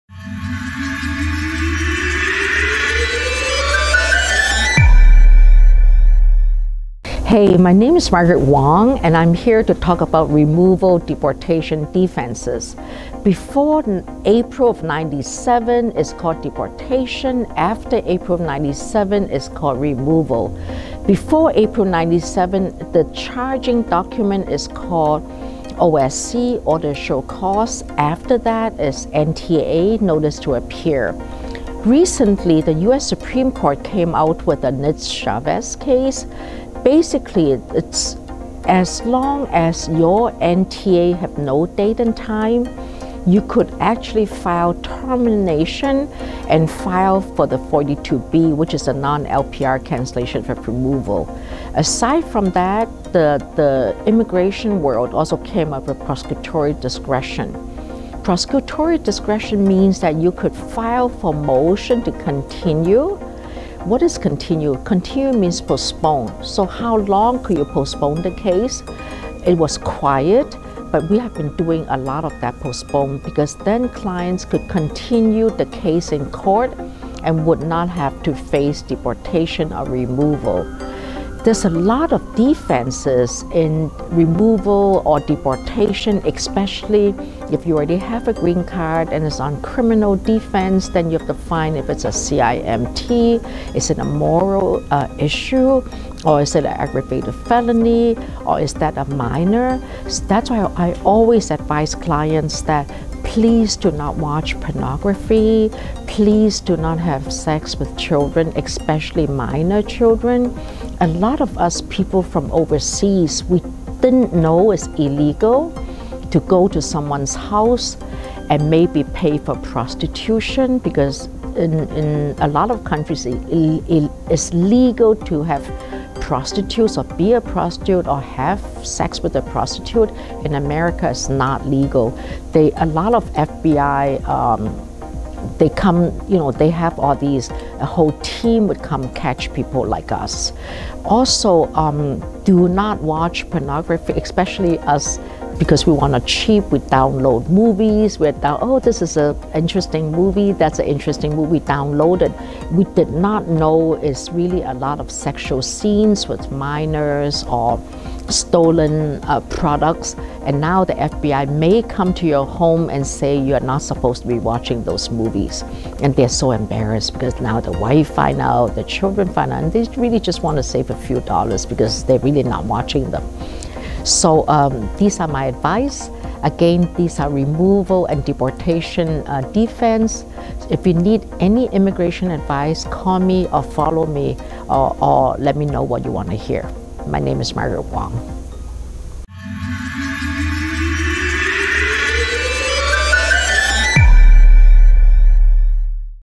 How to defend against DEPORTATION and REMOVAL | Immigration Lawyer Explains All | Margaret W. Wong
Here's how to fight and win against deportation from the point of view of an immigration attorney. Margaret W. Wong explains all.

for more about what we do at Margaret W. Wong & Associates.